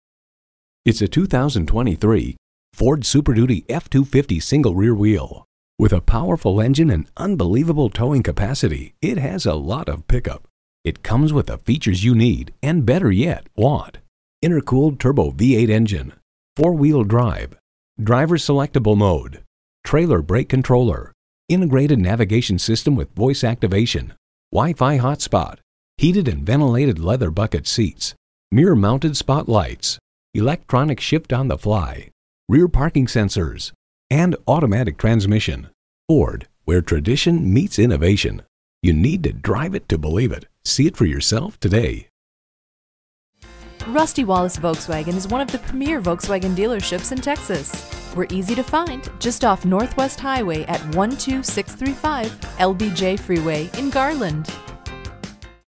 Used 2023 Ford F-250SD Dallas TX Garland, TX PD83074 - SOLD
Year: 2023
Make: Ford
Model: F-250SD
Trim: Lariat
Engine: Power Stroke 6.7L V8 DI 32V OHV Turbodiesel
Transmission: 10-Speed Automatic
Color: Black
Interior: Baja
Mileage: 33130
Stock #: PD83074
VIN: 1FT8W2BTXPED83074

Address:
12635 Lyndon B Johnson Freeway